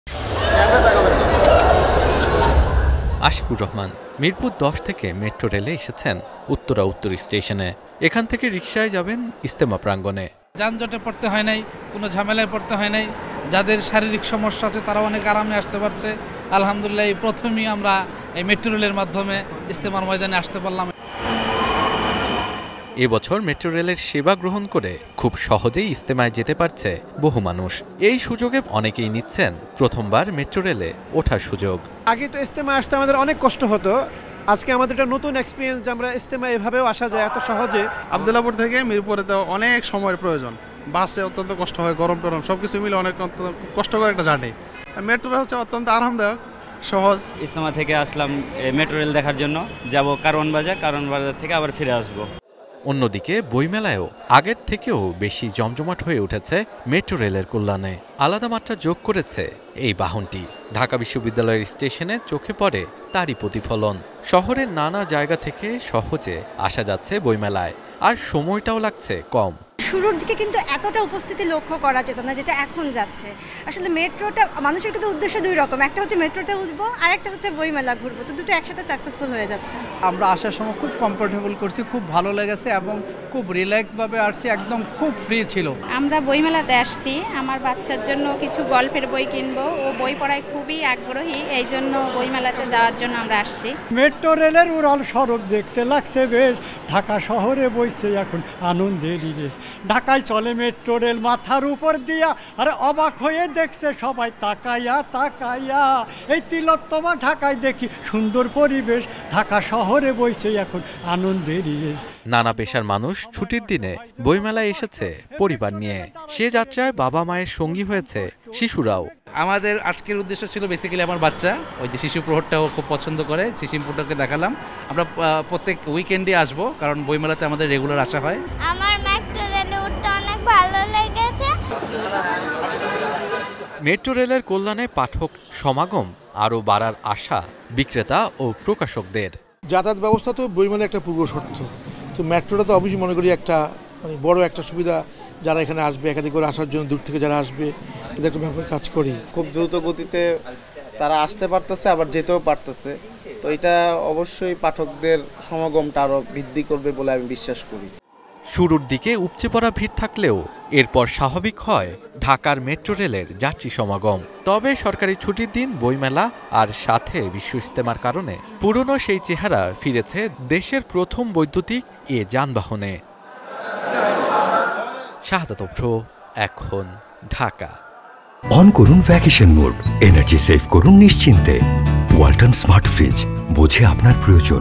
বইমেলা ও বিশ্ব ইজতেমা আরও প্রাণবন্ত করলো মেট্রোরেল | Metro Rail Advantages | Ekhon TV
বইমেলা ও বিশ্ব ইজতেমা যেন আরও একটু বেশি প্রাণবন্ত করলো ঢাকার মেট্রোরেল সেবা। সহজ হয়েছে যাত্রা আর কমেছে সময়। ছুটির দিনে নেই চাকুরিজীবি মানুষের ভিড়। তবে ঢাকা বিশ্ববিদ্যালয় স্টেশন ও উত্তরা উত্তর স্টেশনে দেখা গেছে বইমেলা ও বিশ্ব ইজতেমার যাত্রী।

Our Office Address:
EKHON TV
City Park Lane
19, Hatkhola Road, Wari
Dhaka-1203
Bangladesh